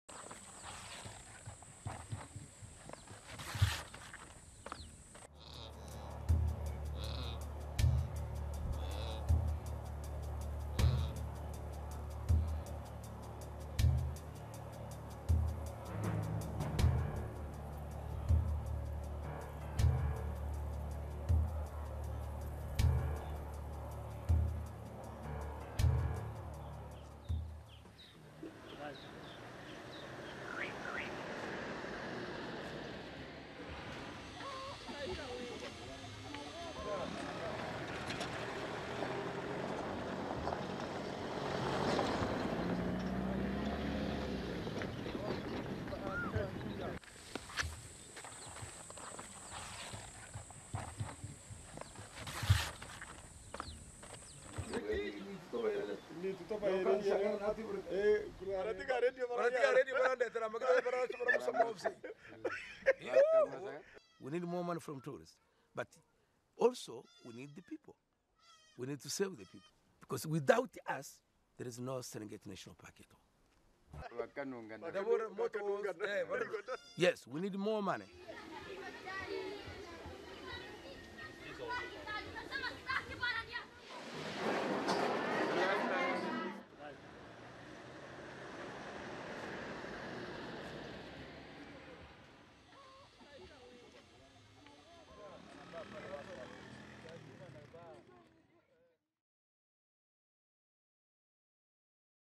Serengeti doğal yaşam parkının geleceği tehlikede - DW Türkçe
Tanzanya’daki Serengeti Milli Parkı milyonlarca vahşi hayvanı barındıran bir doğal yaşam parkı. Ancak Tanzanya halkının ihtiyaçları doğal yaşamı tehdit ediyor. Soruna turizmle çözüm bulundu.